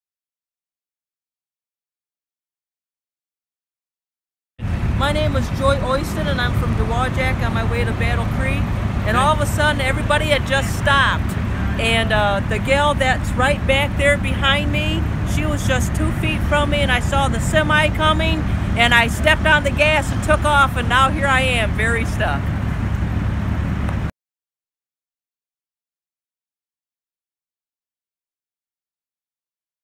Driver describes I-94 pileup near Kalamazoo on Feb. 9
Pile up on I-94 in Kalamazoo, Michigan involved 38 vehicles, according to authorities.